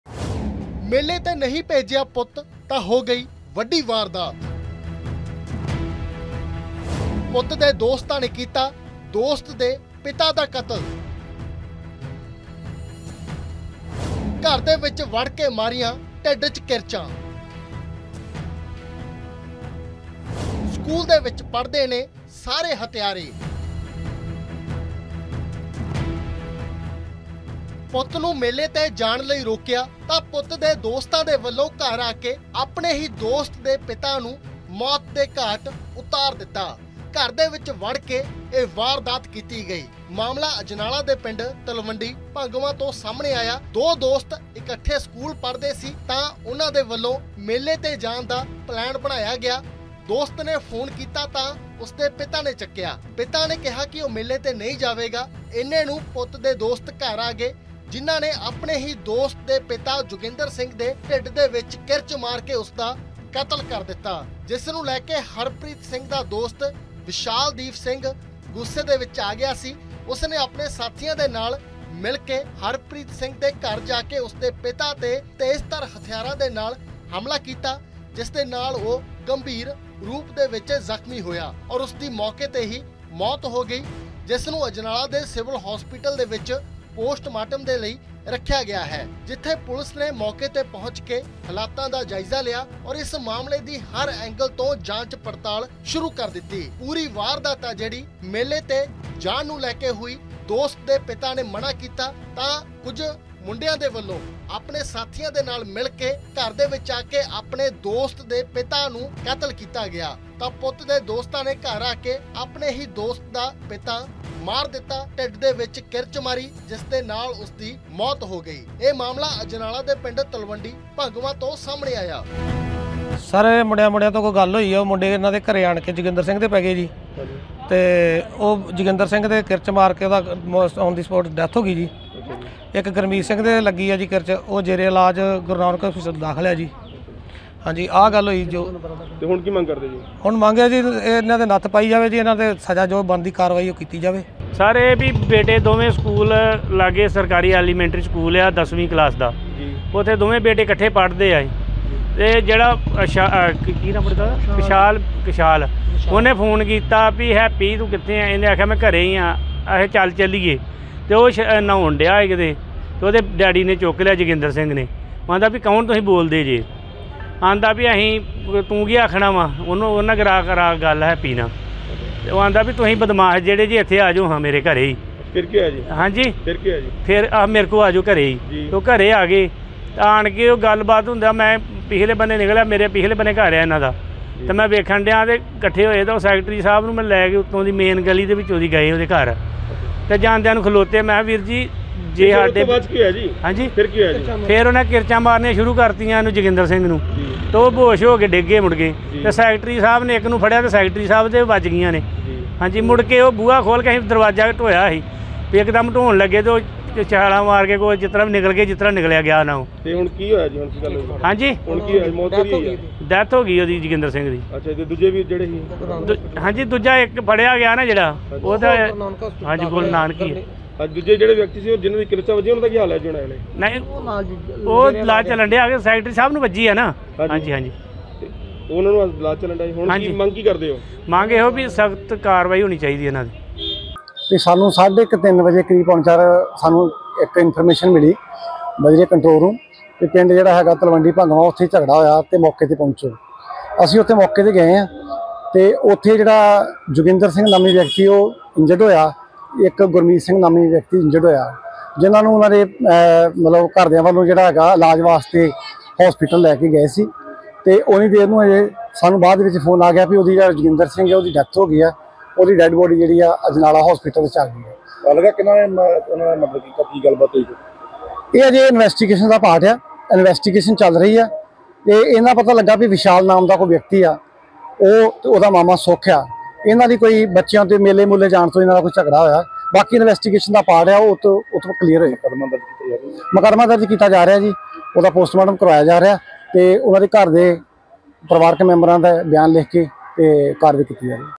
Murder in Amritsar | ਪੁੱਤ ਨੂੰ ਮੇਲੇ 'ਤੇ ਨਹੀਂ ਭੇਜਿਆਂ ਤਾਂ ਪੁੱਤ ਦੇ ਦੋਸਤਾਂ ਨੇ ਕਰ 'ਤਾ ਕਾਰਾ
ਥਾਣਾ ਅਜਨਾਲਾ ਅਧੀਨ ਆਉਂਦੇ ਪਿੰਡ ਤਲਵੰਡੀ ਭੰਗਵਾਂ ਵਿੱਚ ਦੋਸਤ ਨੂੰ ਮੇਲੇ ਭੇਜਣ ਤੋਂ ਰੋਕਣ ਤੇ ਇੱਕ ਦੋਸਤ ਵੱਲੋਂ ਆਪਣੇ ਦੋਸਤ ਦੇ ਪਿਤਾ ਦਾ ਕਿਰਚ ਮਾਰ ਕੇ ਕਤਲ ਕਰ ਦਿੱਤਾ ਗਿਆ। ਜਾਣਕਾਰੀ ਮੁਤਾਬਕ ਵਿਸ਼ਾਲਦੀਪ ਸਿੰਘ ਅਤੇ ਹਰਪ੍ਰੀਤ ਸਿੰਘ ਇਕੱਠੇ ਦੋਸਤ ਹਨ ਅਤੇ ਇੱਕੋ ਹੀ ਸਕੂਲ ਵਿੱਚ ਪੜ੍ਹਦੇ ਹਨ।